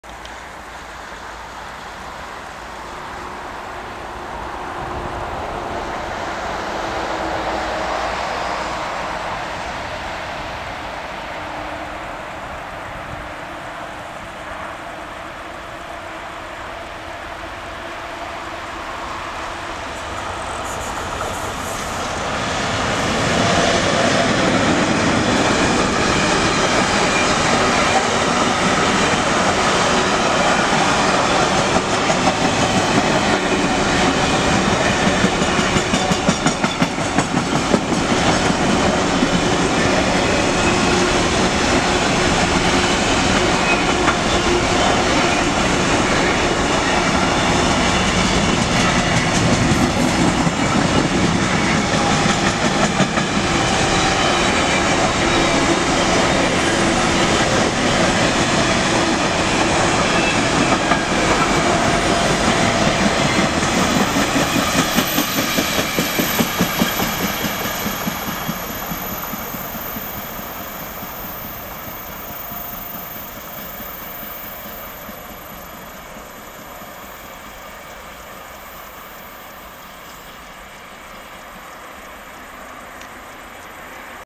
"Biała Dama" ze składem kontenerów na linii 62
Skład nagrany koło Chrząstowic Olkuskich